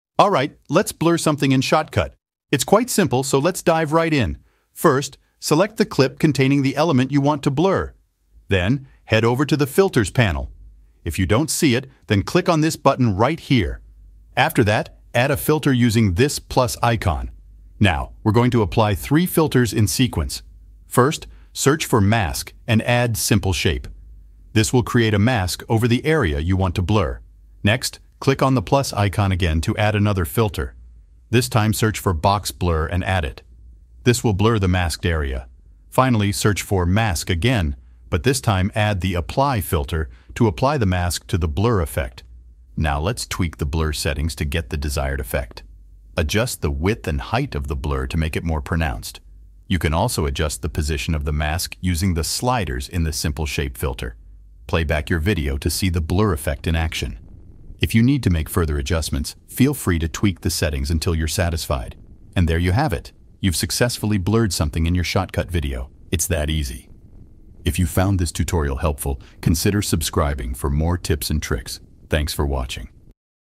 How to Blur Something in Shotcut
In this short tutorial I show you how to blur face in Shotcut easily in just a few simple steps.